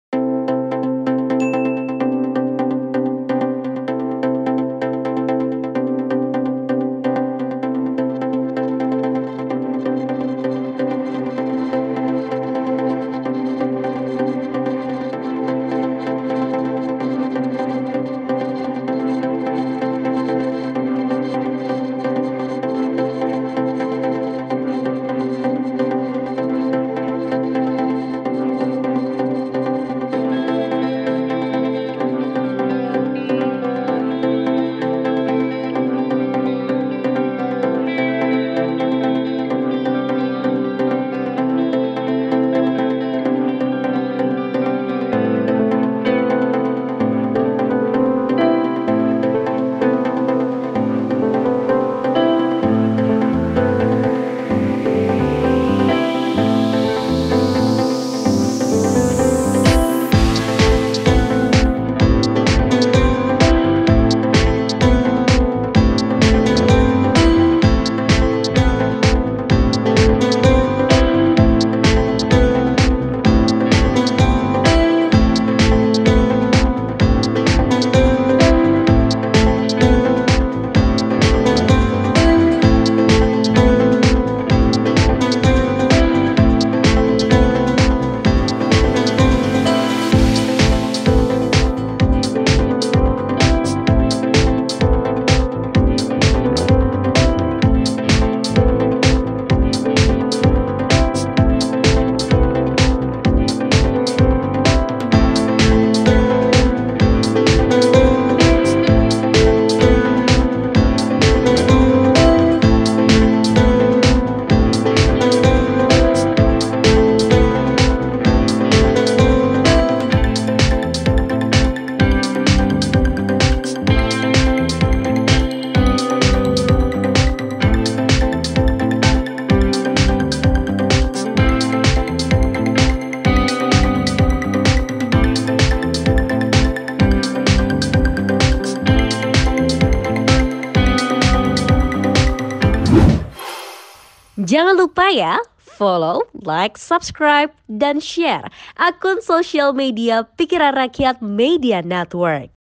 Detik-detik Polisi Tolong Ibu Hamil Kelelahan Naik Motor saat Mudik, Lawan Arus ke RS PMI Bogor
Video aksi heroik polisi menolong ibu hamil yang tak kuat melanjutkan perjalanan mudik 2022 sehingga harus digotong dan dibawa ke Rumah Sakit (RS) PMI Kota Bogor diunggah akun @satlantas_polrestabogor pada Sabtu, 30 April 2022. Dalam video singkat itu, terlihat anggota Satlantas Polresta Bogor Kota menggotong seorang ibu hamil yang masih memakai helm masuk ke mobil.
Disebutkan sebelumnya, ibu hamil yang tidak diketahui identitasnya itu datang ke Pos Pam Terpadu Operasi Ketupat Lodaya 2022 di Baranangsiang. Tampak tubuhnya dalam kondisi lemas.Hingga akhirnya anggota bernama Ipda Desty, Bripka Agus Prio, dan Briptu Nina membantu ibu hamil yang kelelahan itu menuju RS PMI Bogor untuk segera mendapatkan pertolongan medis yang tepat.
Agung Tri Nurcahyo/ @prfmnews
Vid. Editor: Gesang/PRMN